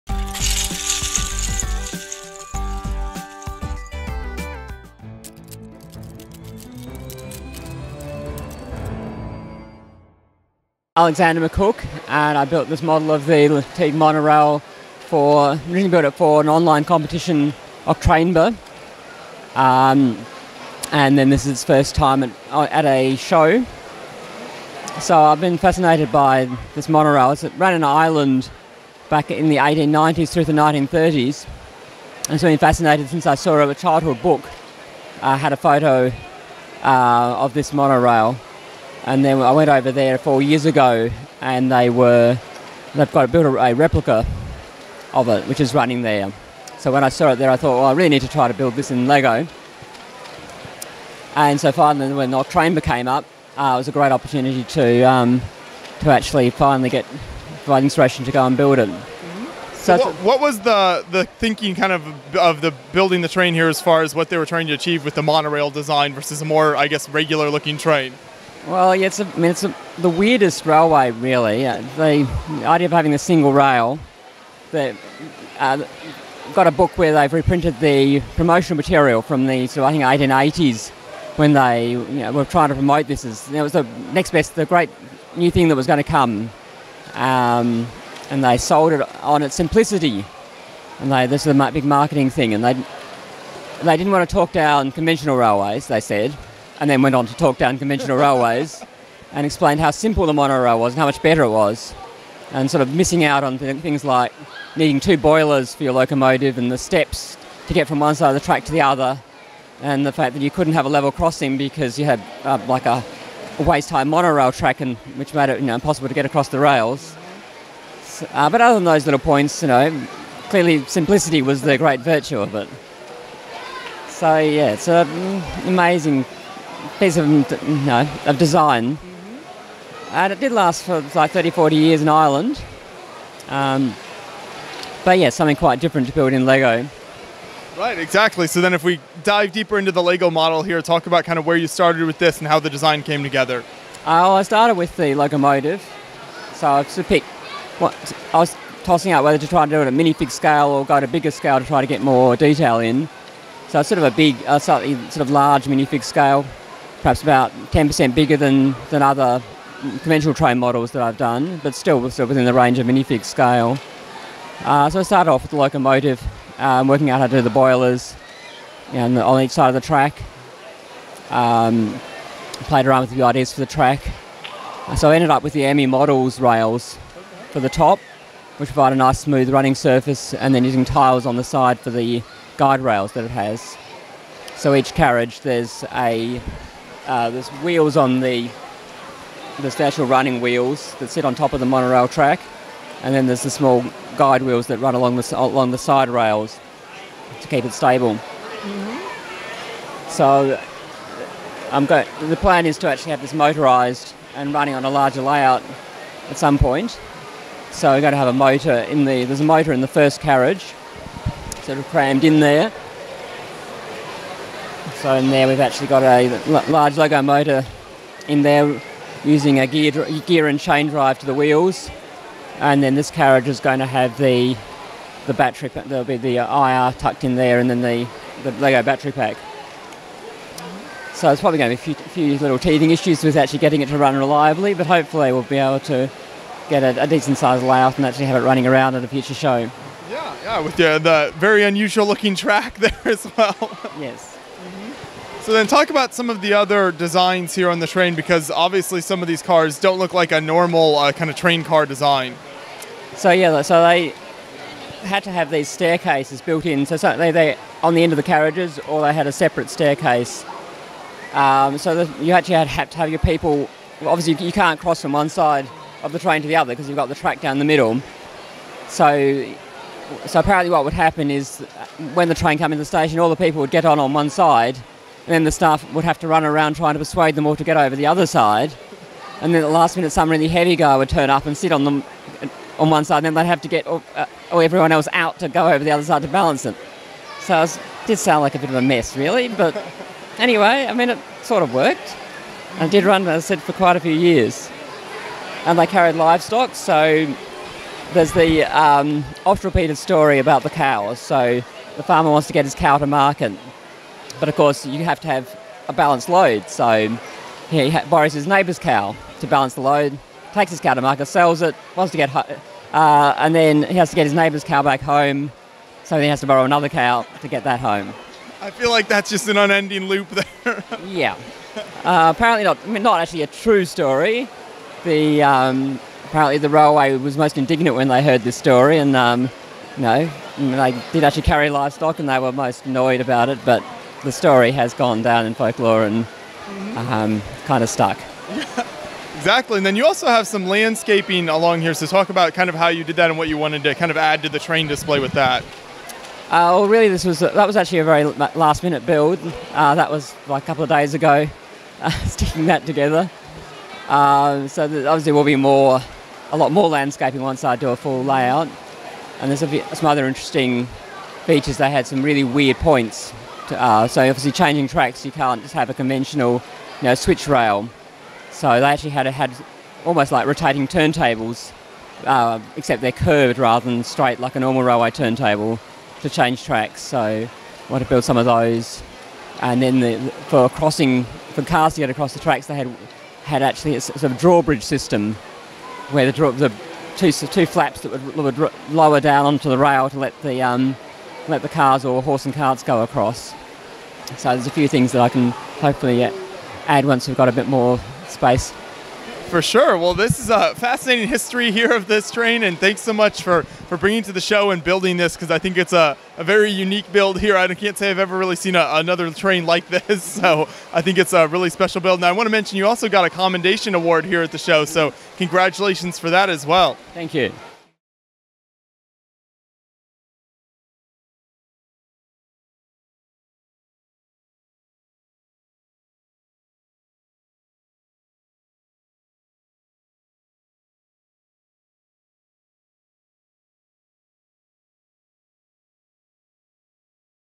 LEGO Lartigue Monorail Locomotive | Brickvention 2019
Beyond the Brick's Joshua Hanlon talks with Alexander McCooke about his LEGO Lartigue Monorail at Brickvention 2019 in Australia.